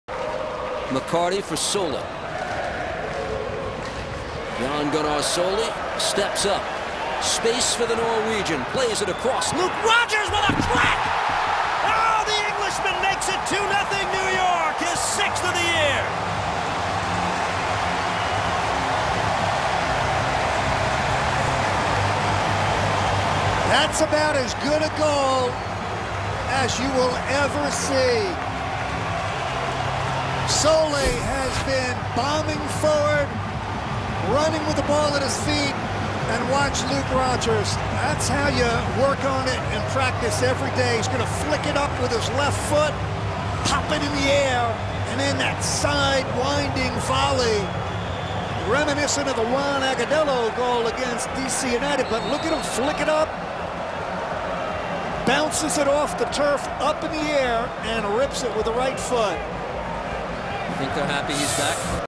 RBNY's Luke Rodgers scores from a classy volley
New York's Luke Rodgers receives the ball from Jan Gunnar Solli, touches it for a self-volley, and smashes in a stunning goal to put the Red Bulls up 2-0.